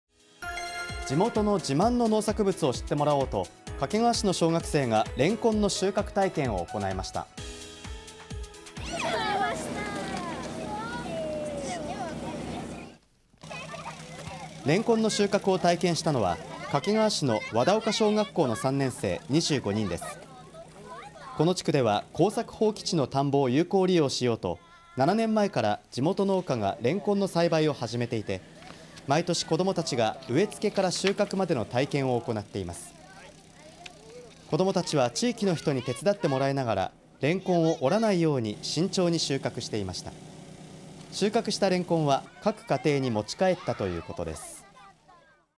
小学生がレンコン収穫体験 耕作放棄地活用と地元農産物を知ってもらう取り組み 静岡・掛川市 ２日
地元の自慢の農作物を知ってもらおうと、静岡・掛川市の小学生がレンコンの収穫体験を行いました。